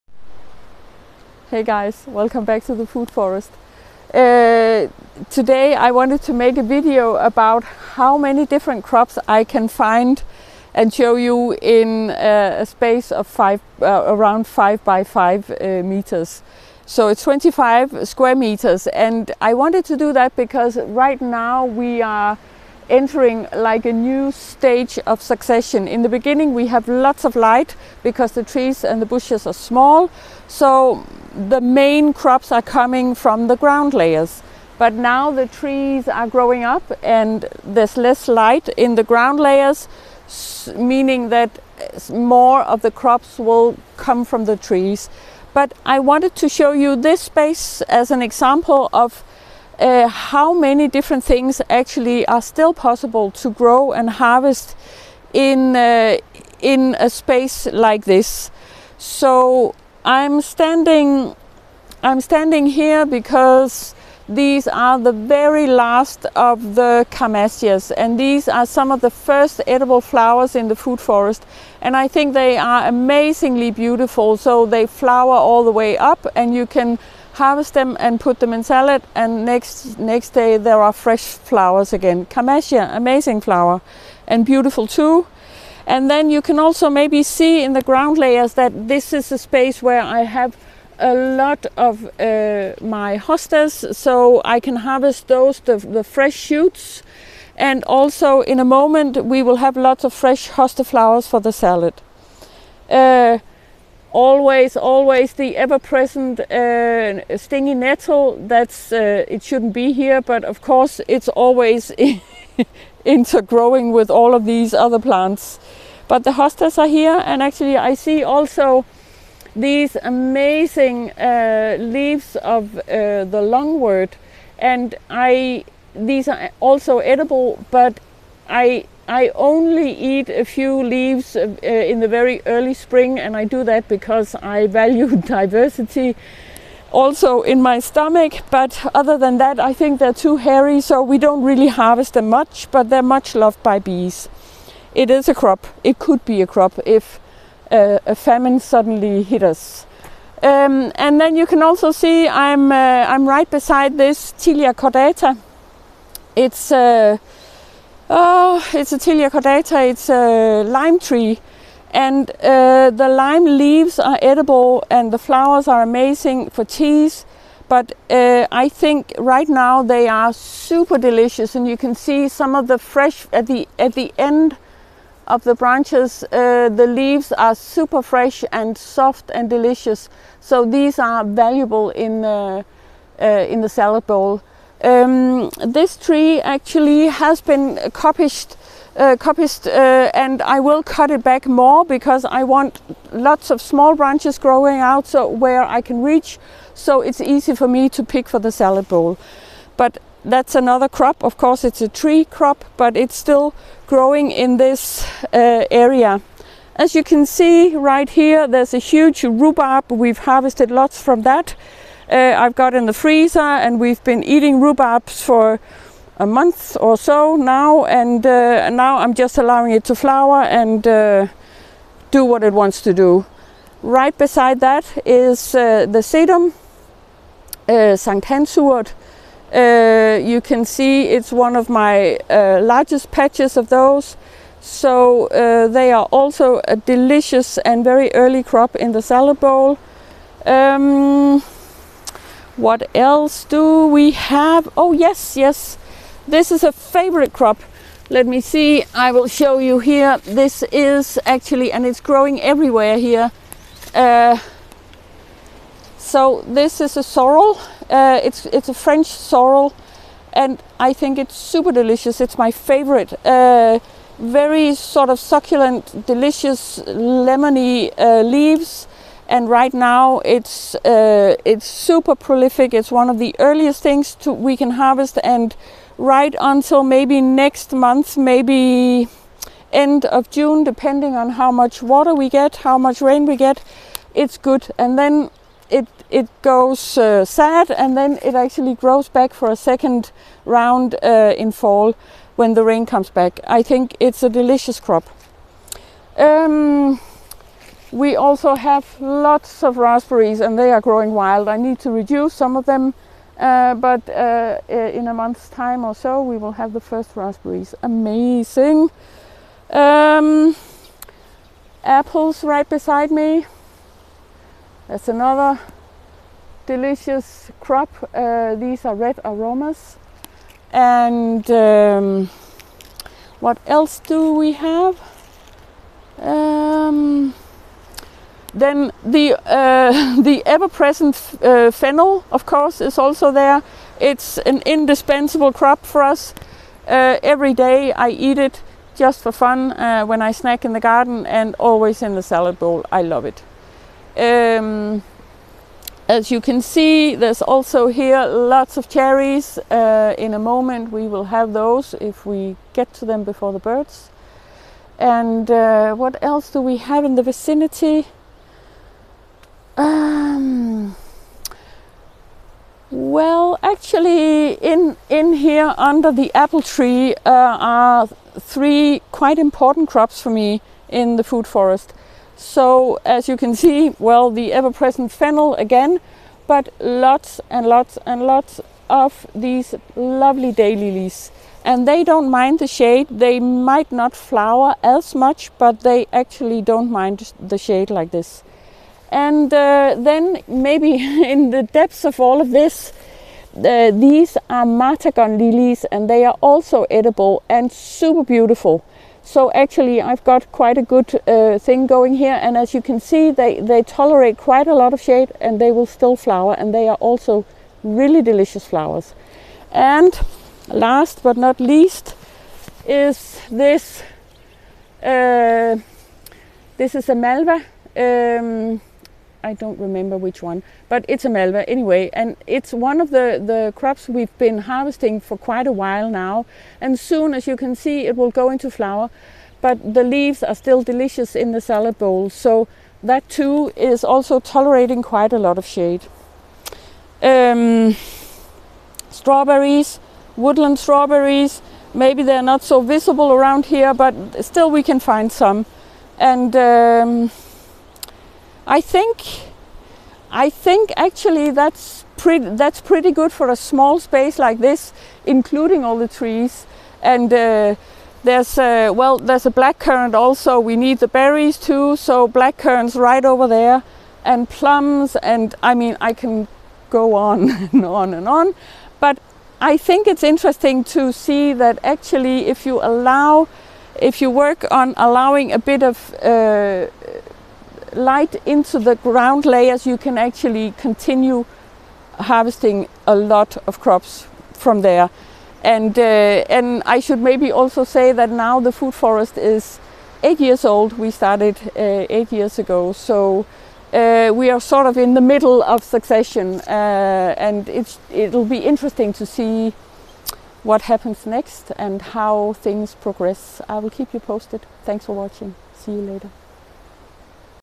19 different crops in a 5x5 m Space in The Food Forest! Permaculture Nordic Food Forest
The Food Forest is now around 8 years old, so we are approximately in the middle of succession - meaning that most of the trees will be full grown in another 7-8 years. It is interesting to follow how the changing growing conditions determine what grows where. In this 5 x 5 meter space I found the following crops:
Camassia
Tilia Cordata/Lime
Hosta
Sorrel - de Belleville
Fennel
Stingy nettle
Woodland strawberries
Strawberries
Martagon lilies
Daylilies
Rhubarb
Sedum
Malva
Apples
Cherries
Eleagnus umbellata
Black Currants
Rosemary
Raspberries
Lungwort